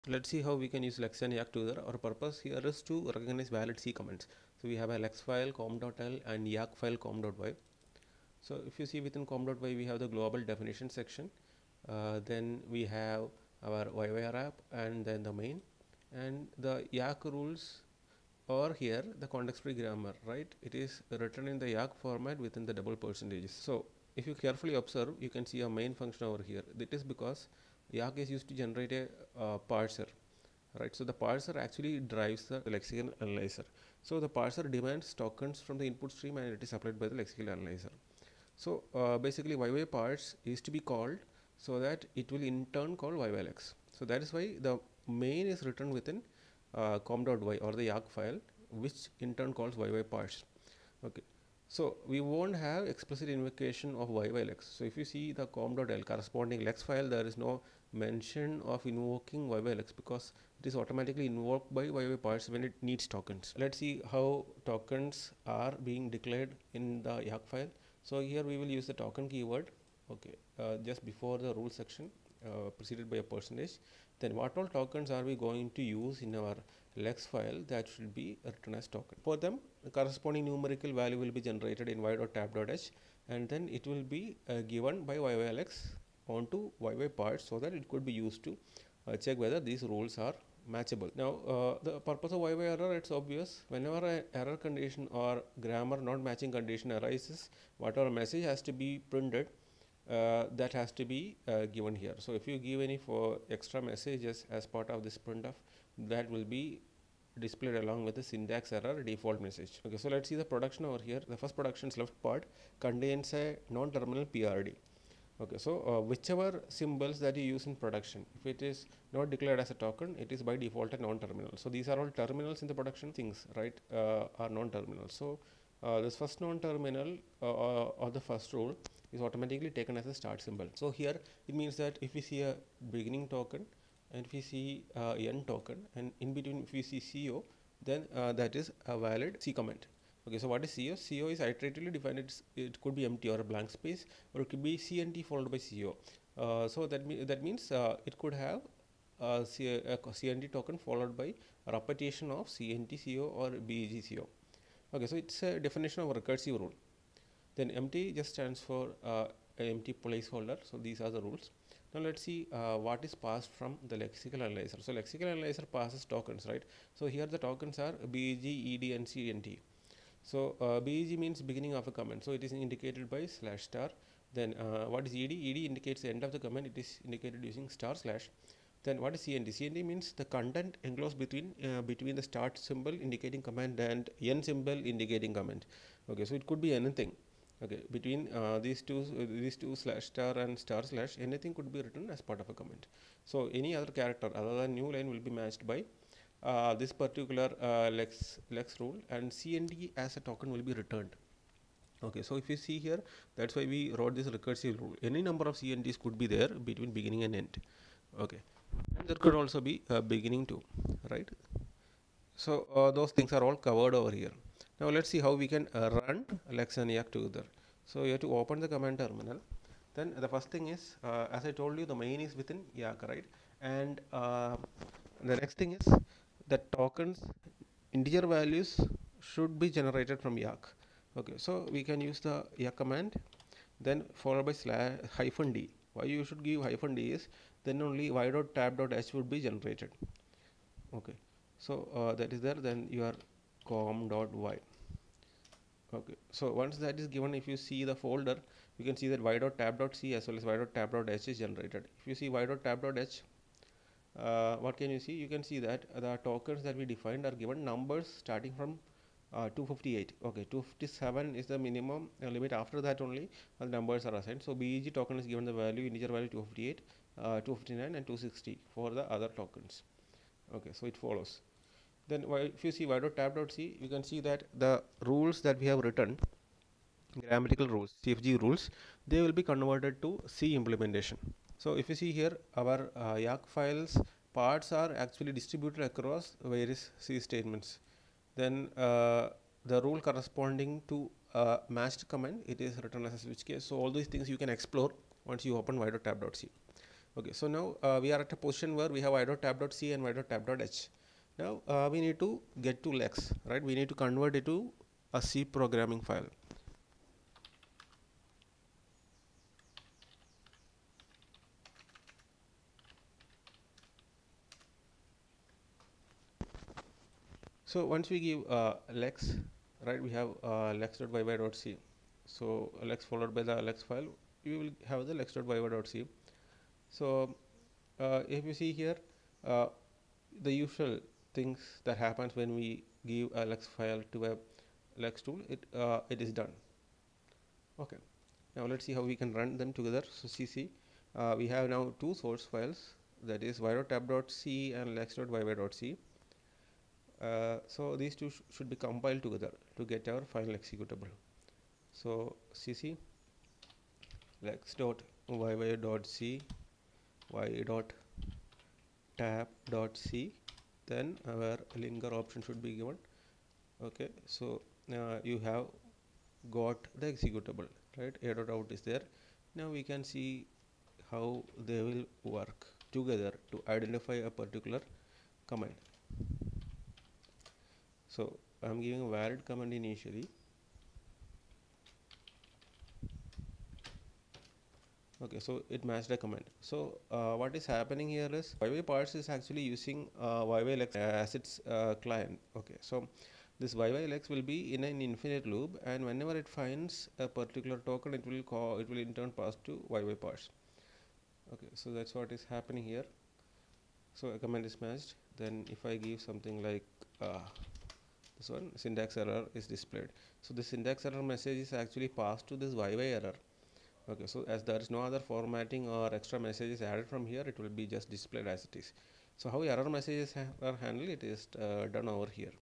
Hands on Yacc--Using Lex and Yacc to Create a Parser (Beginner Level)
Creating a parser using Yacc to identity C programming language comments

(Lex tool is used to generate scanner)